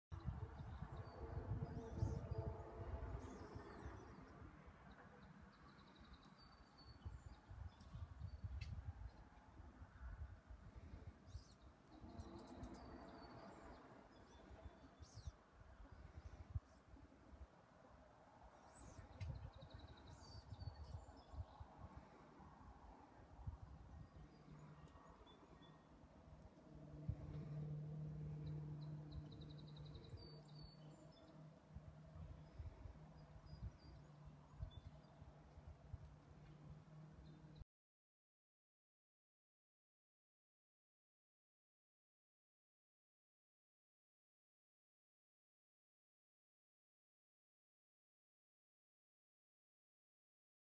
A short of Sir Riley the border collie and Chip the goose!
Outside enjoying spring time weather and playing with the toys!